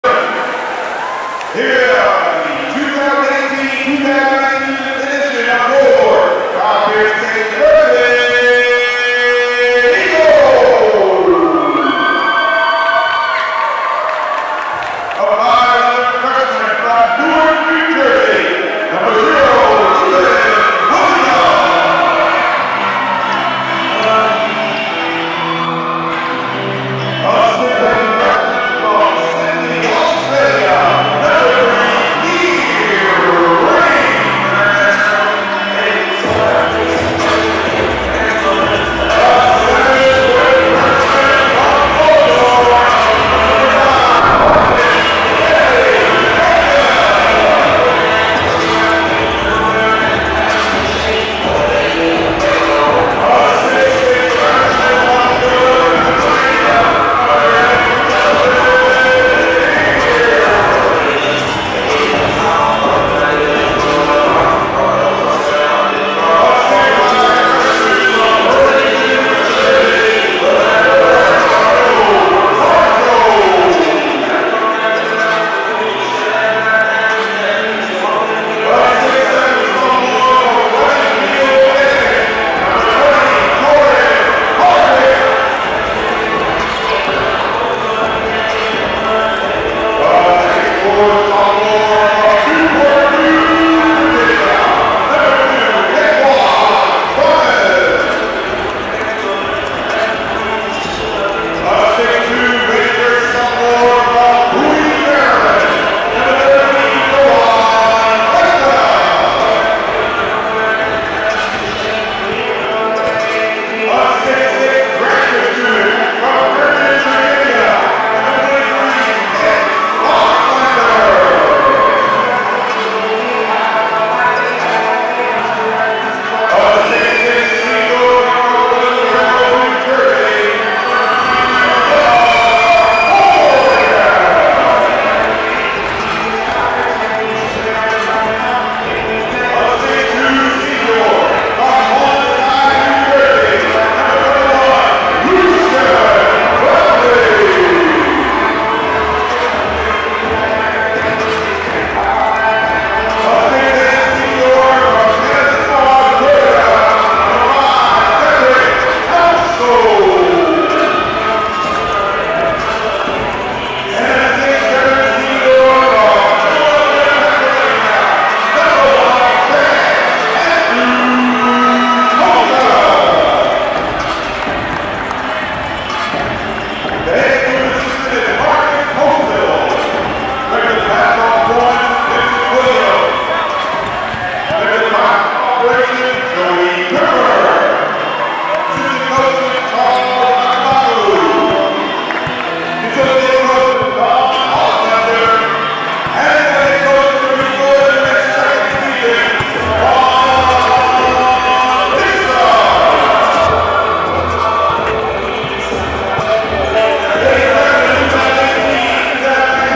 Midnight Mayhem 2018: 2018-19 Coppin State Eagles Team Intro
First introduction to Coppin State Men's Basketball for 2018-19